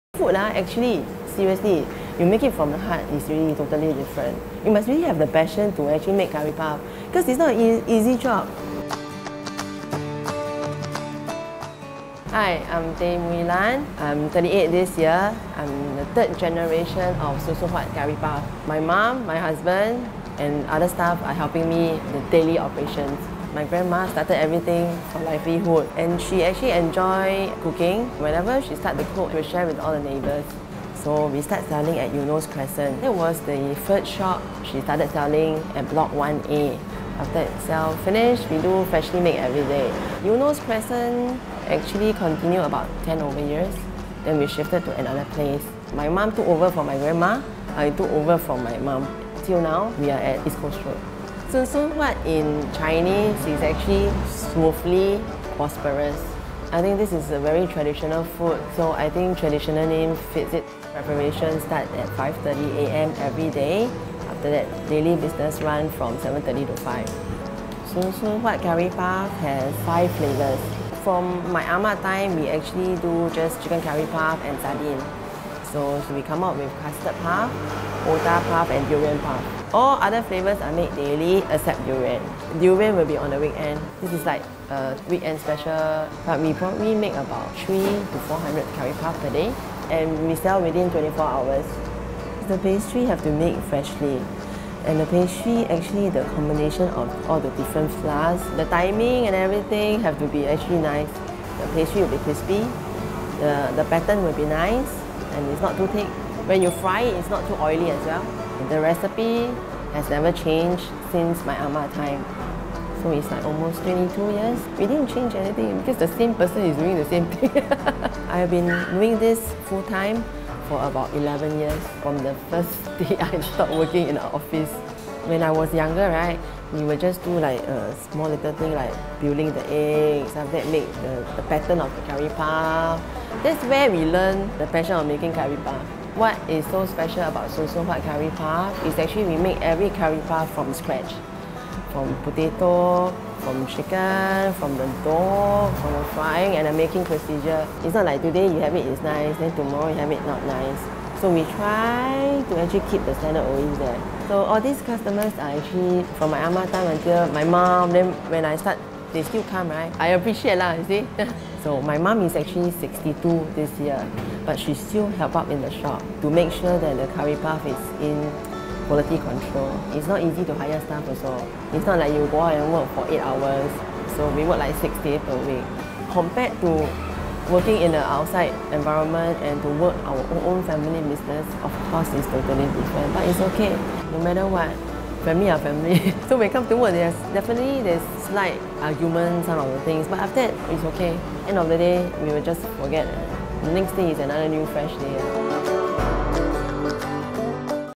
Handmade Crispy Curry Puffs From Scratch Soon Soon Huat Curry Puffs
Hidden in a corner along East Coast Road, Soon Soon Huat Curry Puff prides itself as one of Singapore’s pioneer curry puff makers.

Previously located at Blk 1A Eunos Crescent and formerly known as ‘1A Crispy Curry Puff’, the owners of Soon Soon Huat Curry Puff have been in the pastry industry for nearly three decades.

Every working day, the mother-daughter duo, 38-year-old Tay Mui Lan and 60-year-old Ow Siew Kheng, arrive early to prepare the ingredients and hand-make the curry puffs from scratch.

Soon Soon Huat Curry Puff
220 East Coast Road,
Singapore 428917